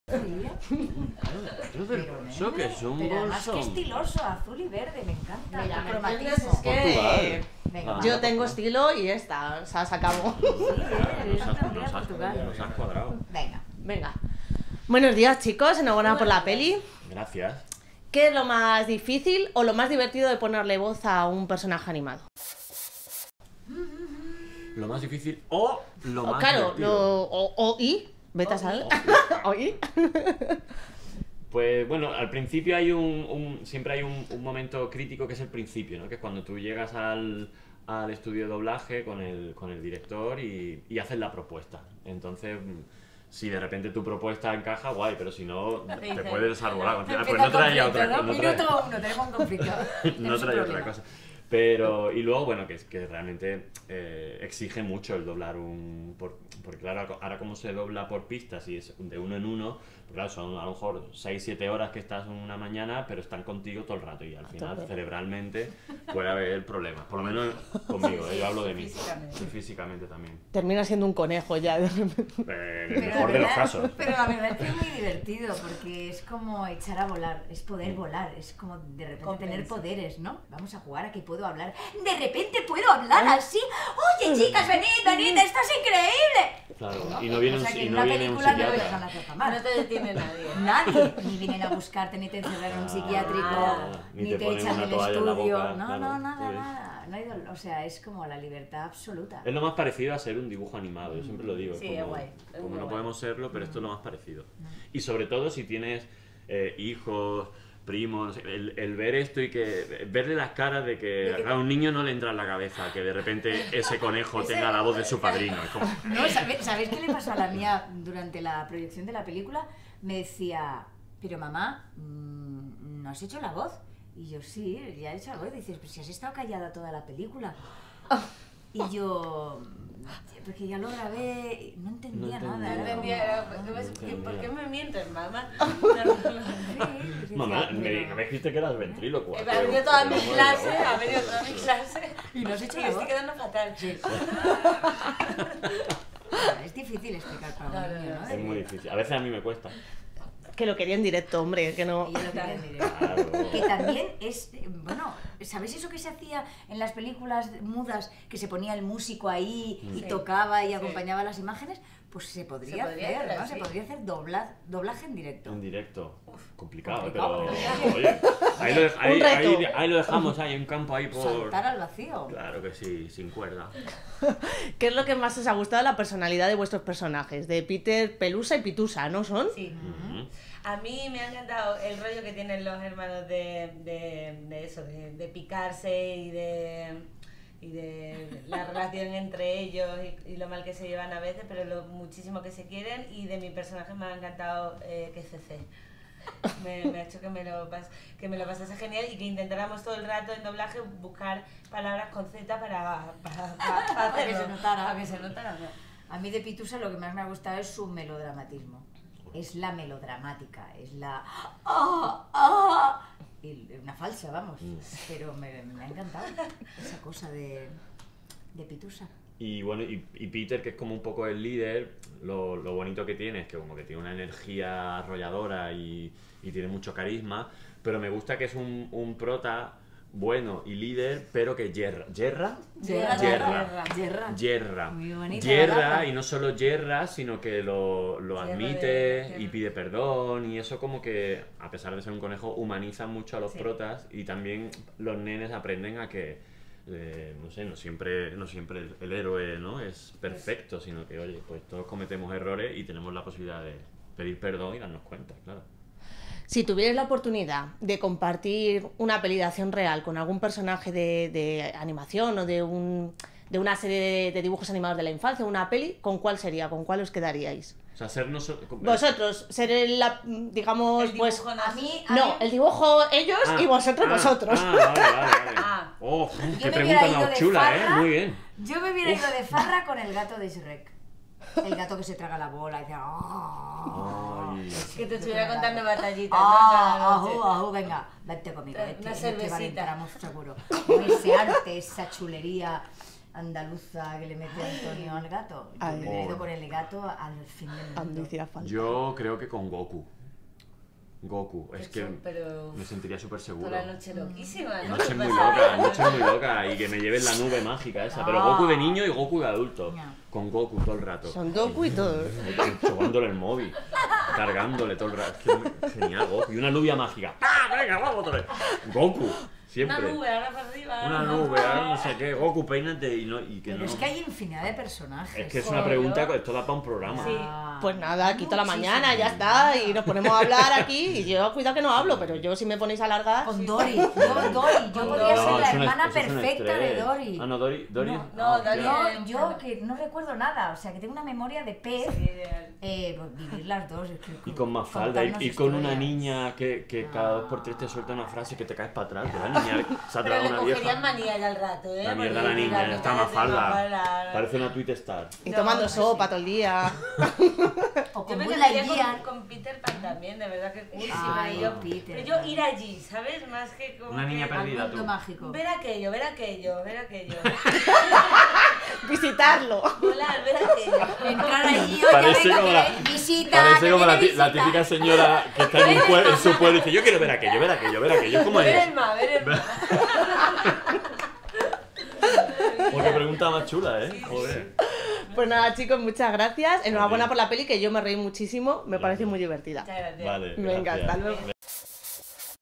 Peter Rabbit: Silvia Abril, Dani Rovira y Belén Cuesta (entrevista)
La película Peter Rabbit (2018) llega a la cartelera el viernes 23 de marzo de 2018. Con motivo de su estreno entrevistamos en exclusiva a las voces españolas de Peter, Pelusa y Pitusa: Dani Rovira, Silvia Abril y Belén Cuesta. Hablamos con ellos de lo más divertido y lo más complicado de doblar a un personaje animado, de con qué personaje de animación compartirían protagonismo en una película... Más cine

Sinopsis de la película "Peter Rabbit" (2018): Peter Rabbit es un conejito vivaracho, inteligente e ingenioso capaz de todo por entrar a la huerta del malhumorado Mr. McGregor (Sam Neill). Su astucia no será  arma suficiente cuando intente librarse del nuevo inquilino Mr. Thomas McGregor (Domhnall Gleeson) que parece haber conquistado el corazón de su querida amiga Bea (Rose Byrne).